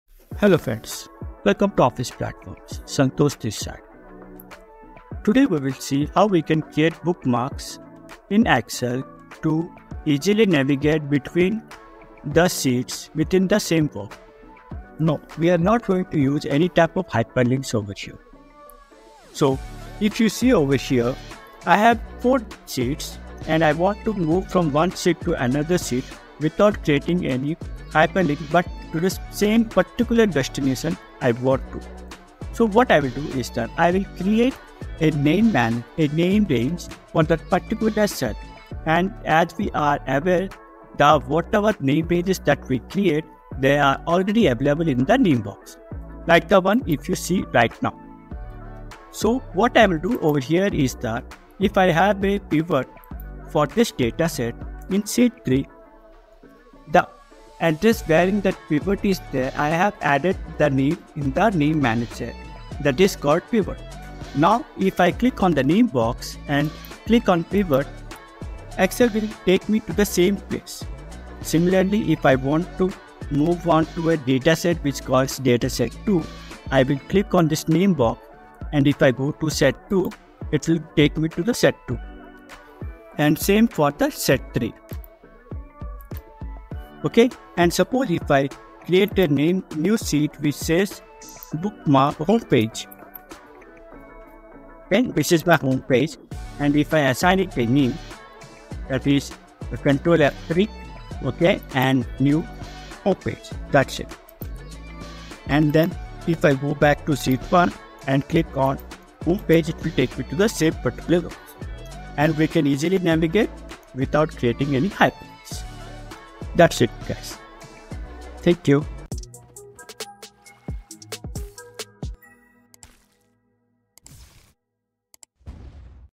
Creating Bookmark in Excel without hyperlinks
In this video we will see how we can use namebox and namemanager in Excel to create bookmarks to easily navigate within the workbook.